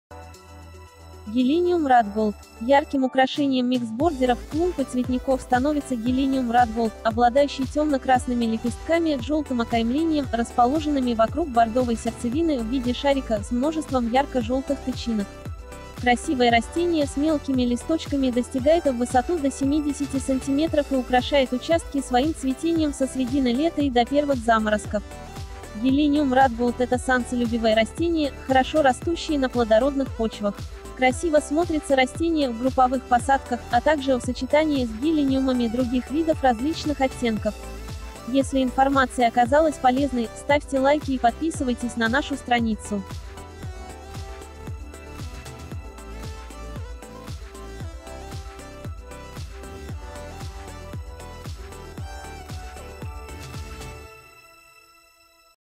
Гелениум Ротголд
Ярким украшением миксбордеров, клумб и цветников становится гелениум Ротголд, обладающий темно-красными лепестками с желтым окаймлением, расположенными вокруг бордовой сердцевины в виде шарика с множеством ярко-желтых тычинок.

Красивое растение с мелкими листочками достигает в высоту до 70 см и украшает участки своим цветением со средины лета и до первых заморозков. Гелениум Ротголд это солнцелюбивое растение, хорошо растущее на плодородных почвах.

Красиво смотрится растение в групповых посадках, а также в сочетании с гелениумами других видов различных оттенков.